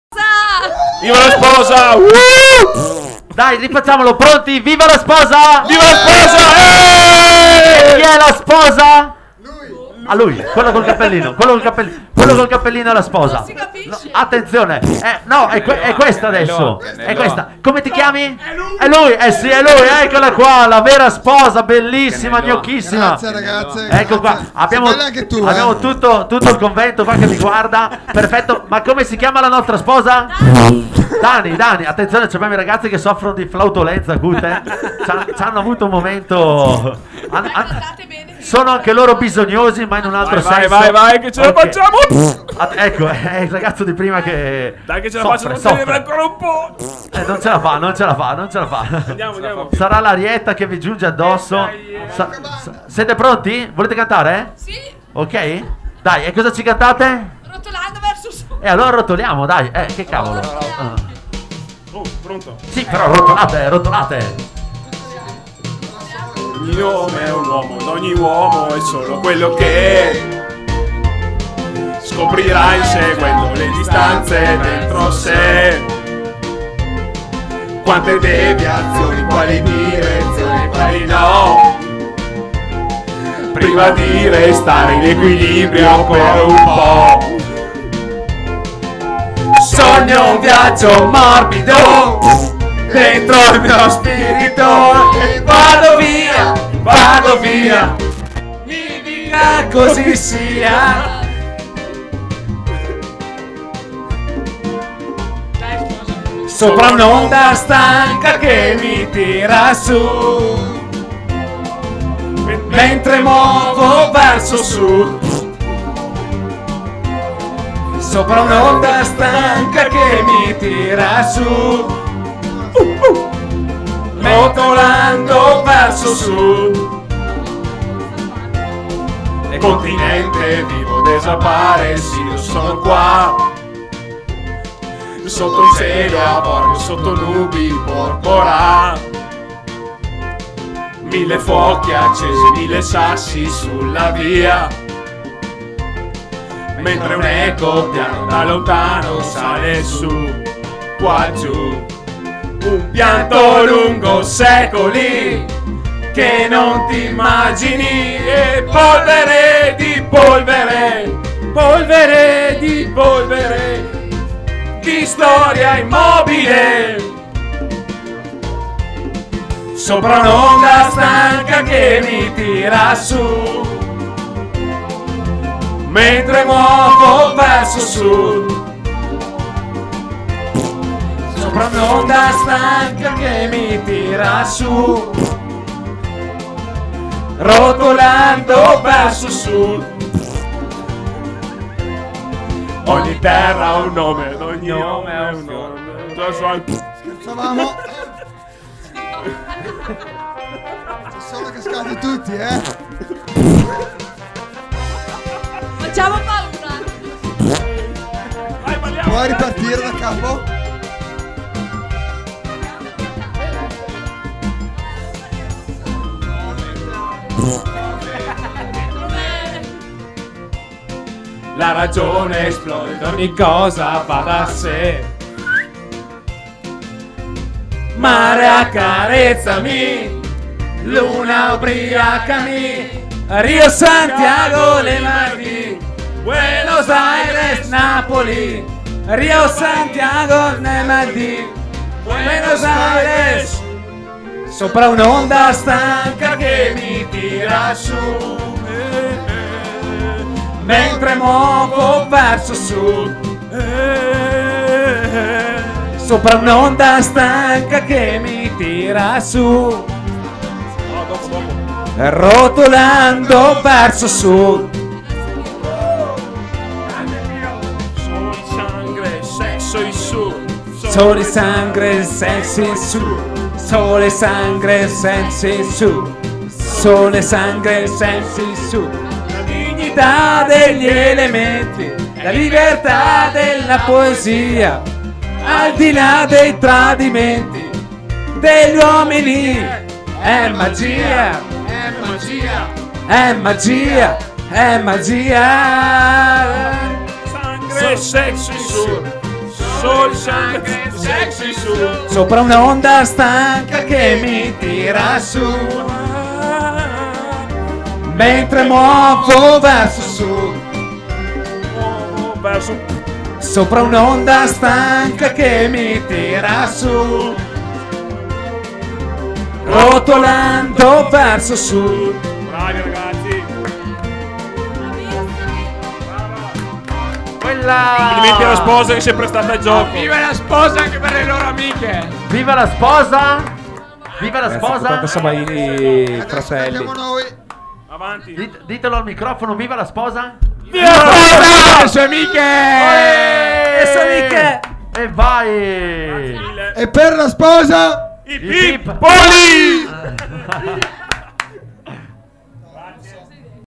Gli amici di Verona che rotolano verso sud al Convento di Lonato Area You Tube sabato 5 maggio
e lascia i tuoi commenti.
sabato 5 maggio
Sabato Dinner'n'Dance
Convento Music Restaurant
 il sito di animazione
Convento Lonato